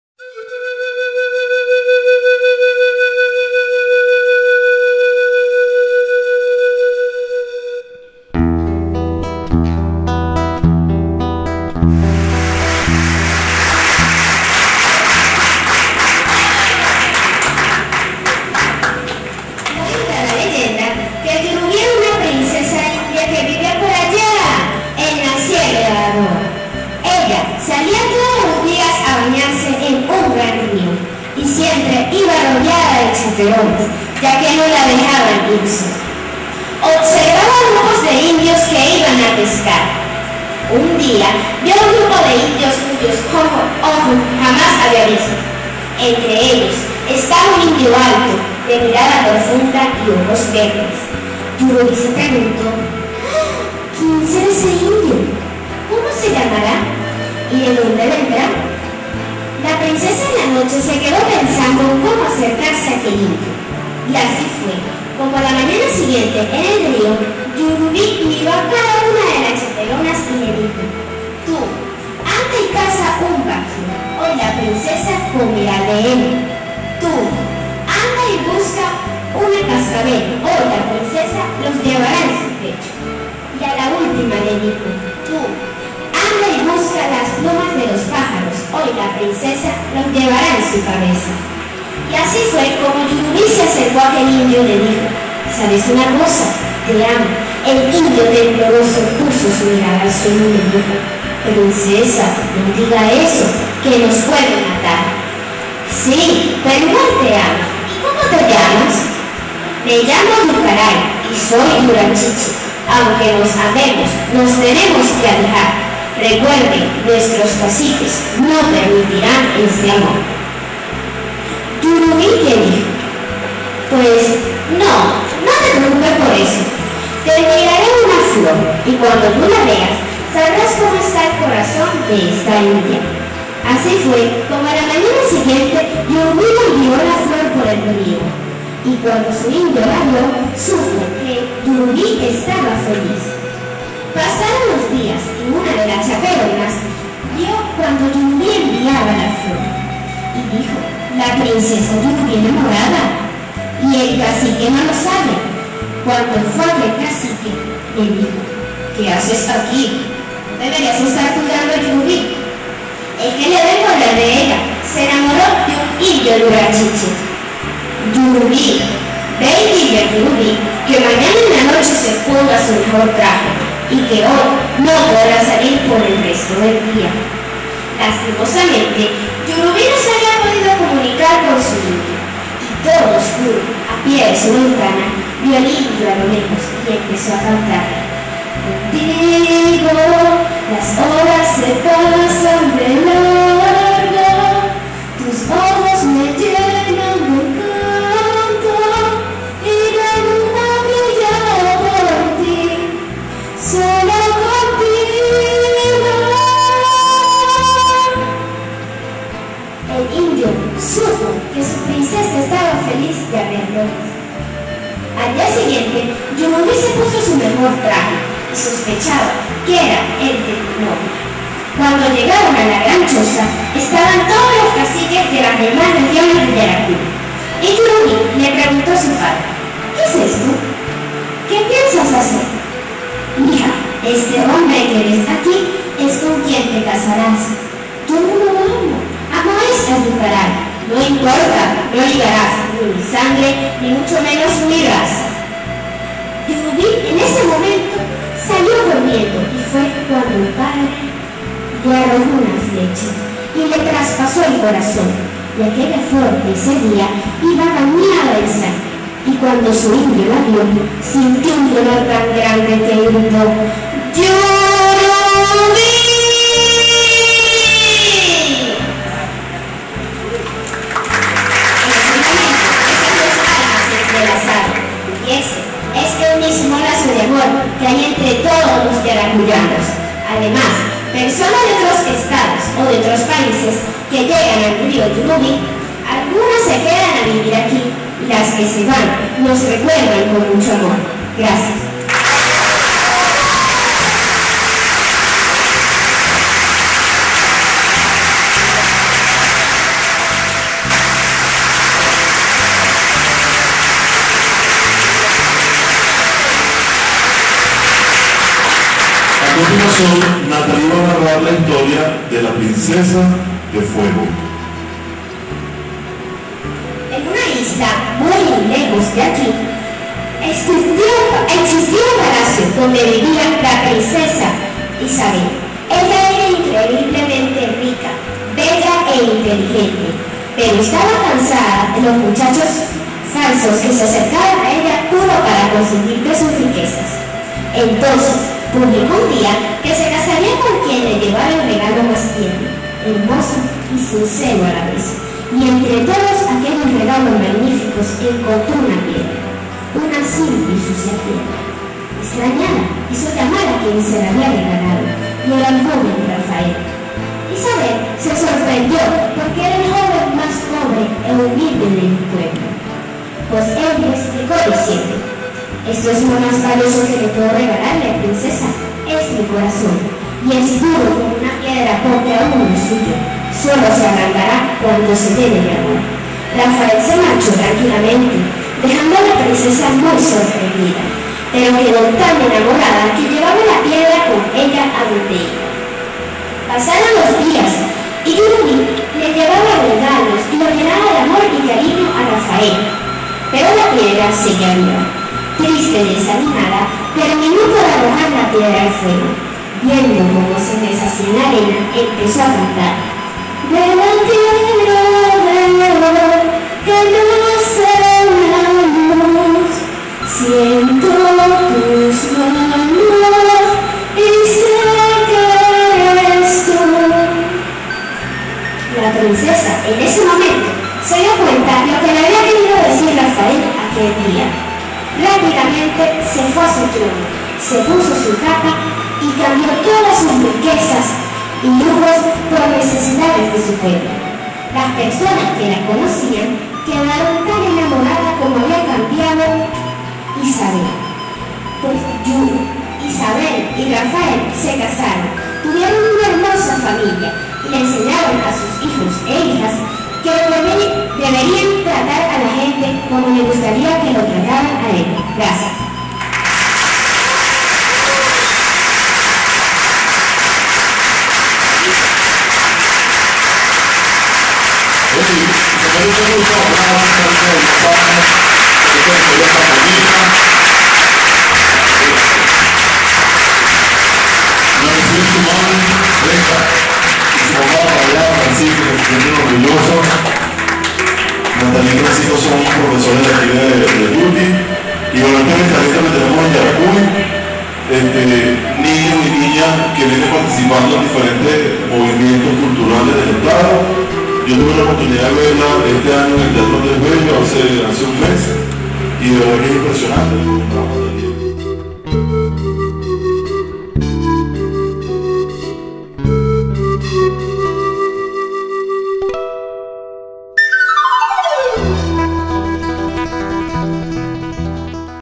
LA LEYENDA DEL YURUBÍ
Contada por la niña Nathaly Coromoto Barrios serrano